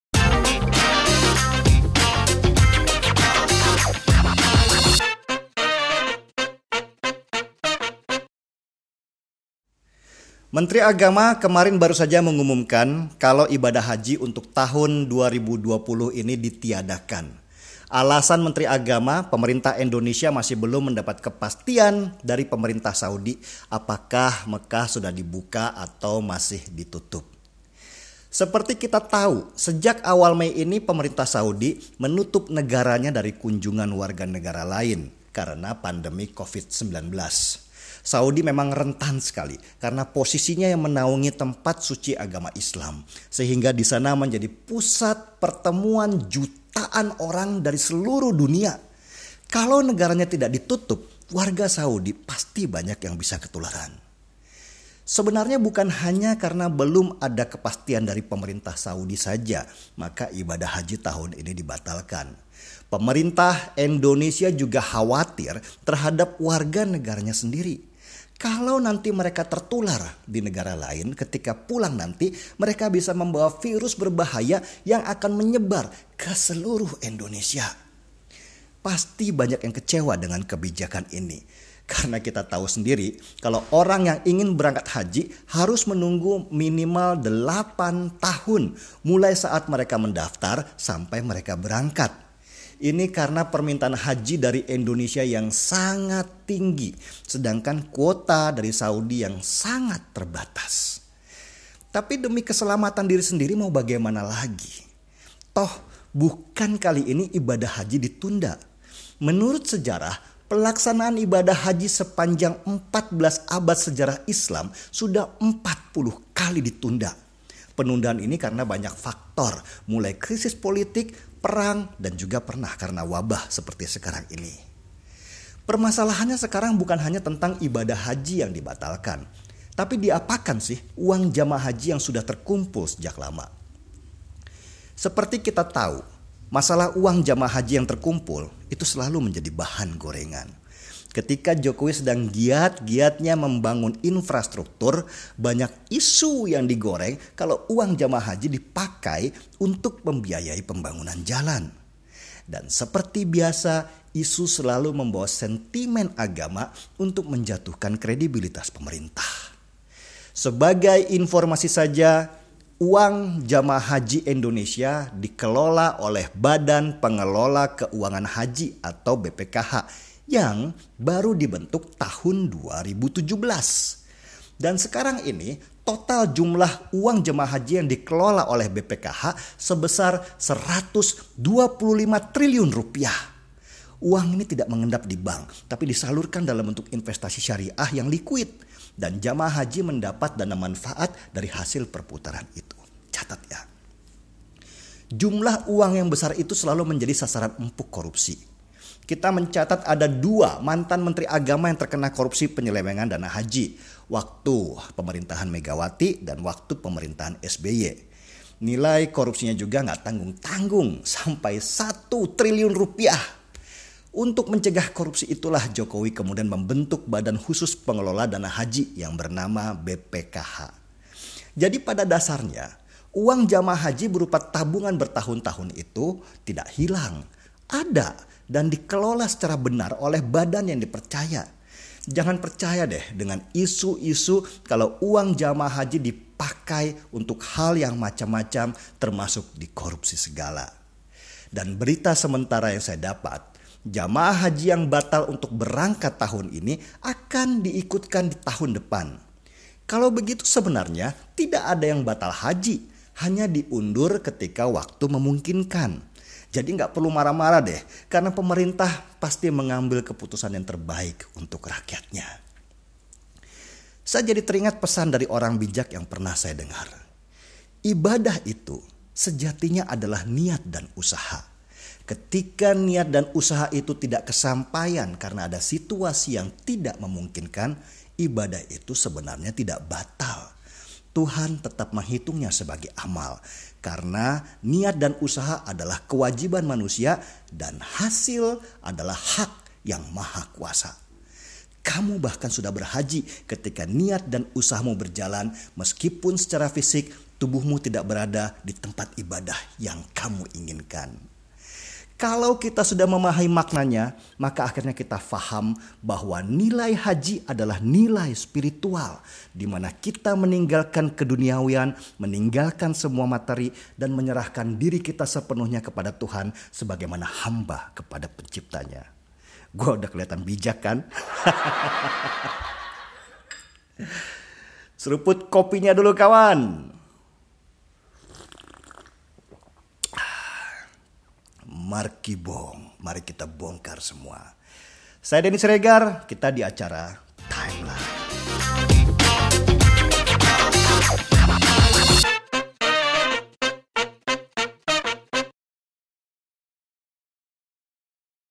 Denny Siregar: HAJI BATAL, UANGNYA KEMANA?
Menteri Agama baru saja mengumumkan jika ibadah haji untuk tahun 2020 ditiadakan karena pemerintah Indonesia belum mendapat kepastian dari pemerintah Saudi. Lalu, kemanakah uang hajinya? Markibong! Simak pemaparan Denny Siregar di Timeline hanya di Cokro TV.